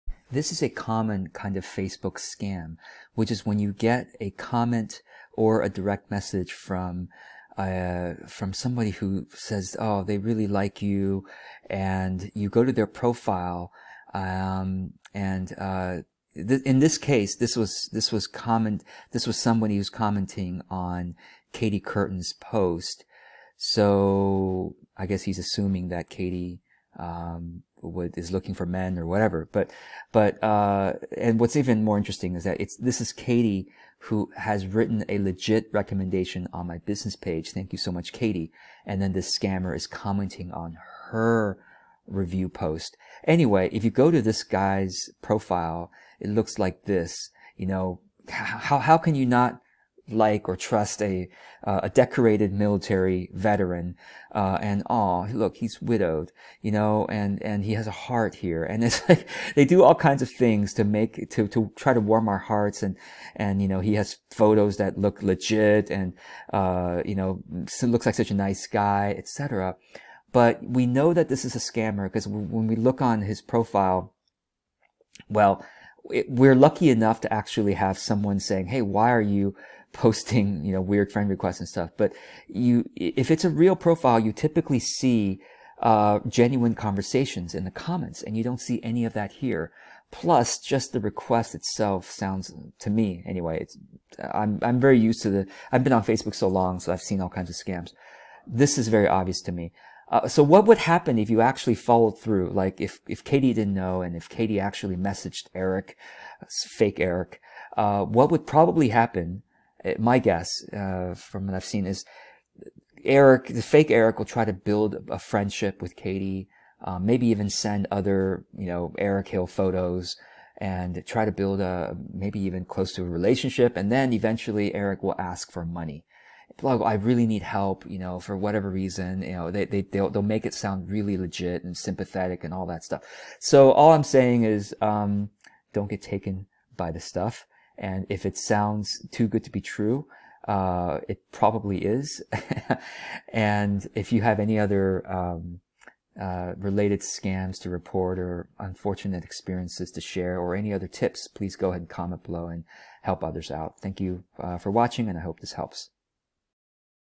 Facebook Scam: fake military looking men who want to friend you...
Don't send them money 😆